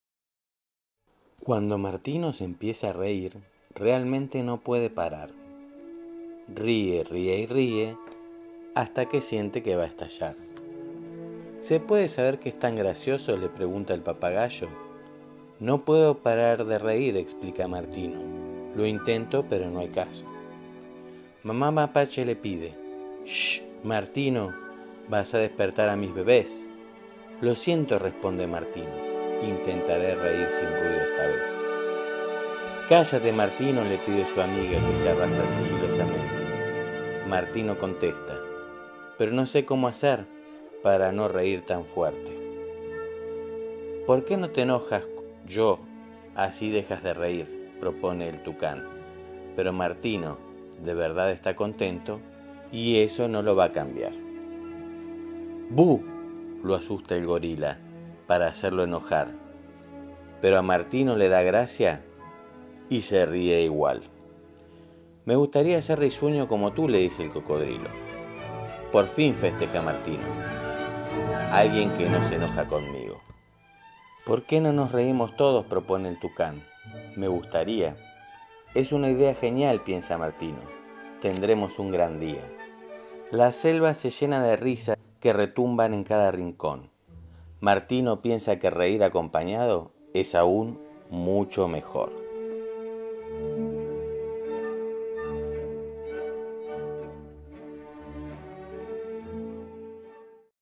El cuento del mono martino
Audiolibro de el mono martino para niños de 2 a 5 años